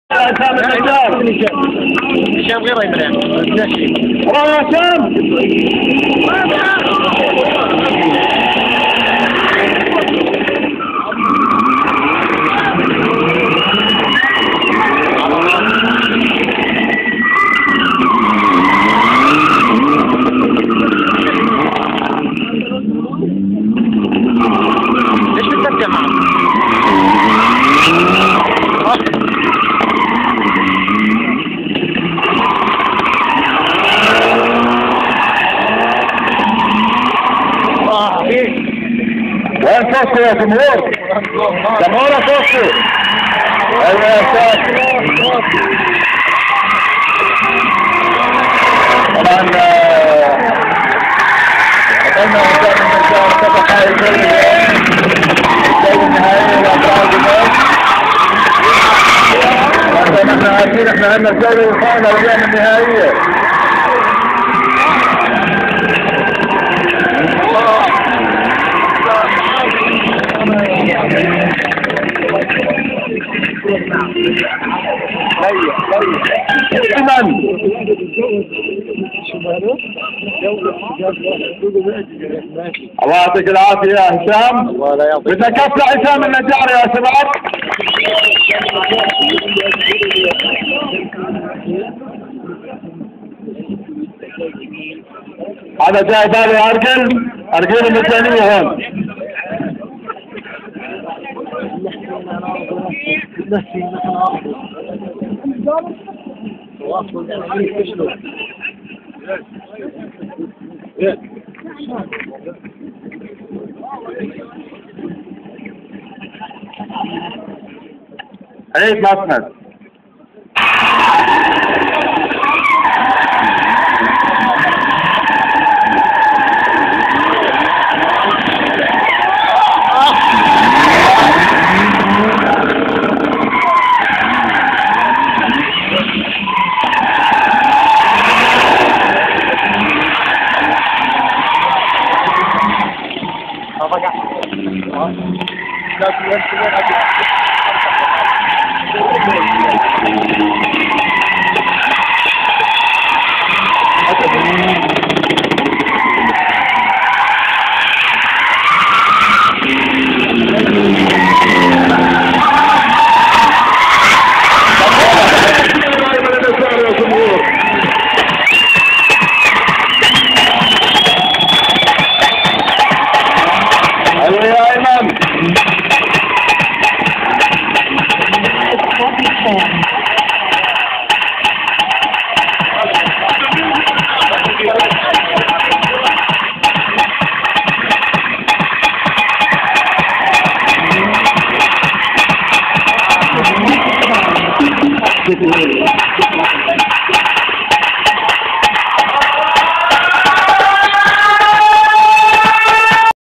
1. Jordan Drifters Event 2-9-2010
Event was held in the Action Target karting track , the event wasnt that big , but the first of its kind in Amman, Jordan